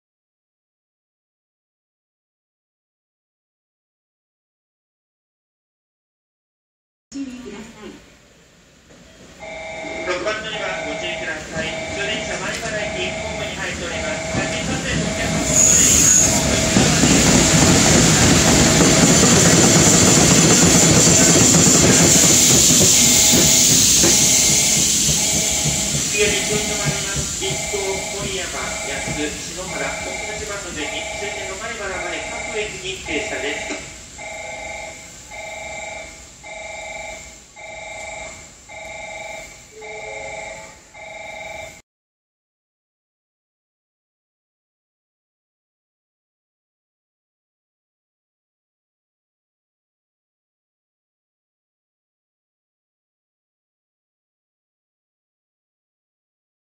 【独特なジョイント音！】EF65-2040号機[新]+シキ800B2+ヨ8625 特大貨物列車9864レ@草津
臨時貨物列車9864レです。牽引機はEF65-2040号機(新鶴見機関区所属・PS17菱形パンタグラフ搭載)でした。シキ800B2の独特なジョイント音が聞きたくて、撮影地をこの駅にしました。他の貨車・釜などでは聞けない16軸装備のシキ800のジョイント音をお楽しみください！
守山駅でも撮影しました。こちらはこれとは違い、爆走シーンが見られます
追記：最初のタイトルの日本語がおかしいので、訂正しました。動画内の字幕はさすがにできないです・・・。